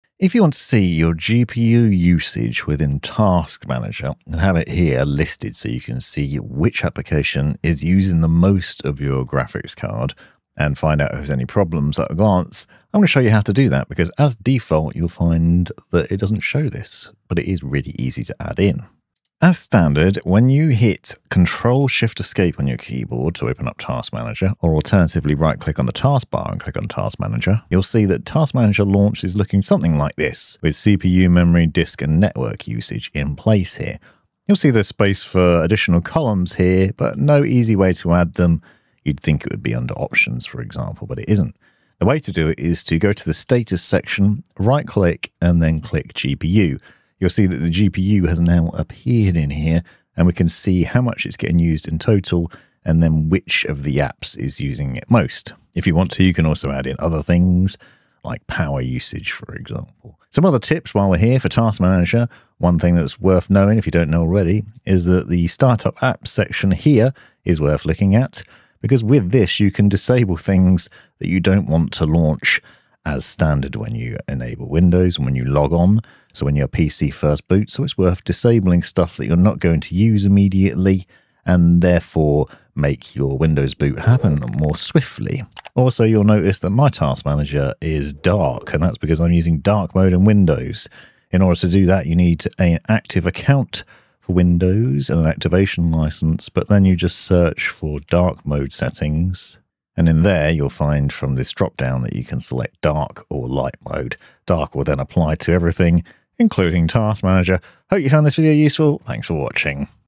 How to see GPU usage in Task Manager and other tips
Prawn here with a couple of hopefully helpful tips for your Task Manager setup in Windows.

Here I'm showing you how to set it up so you can see GPU usage in Task Manager. But also a couple of other things worth knowing too!

MANNINGTREE
CO11 9BE
United Kingdom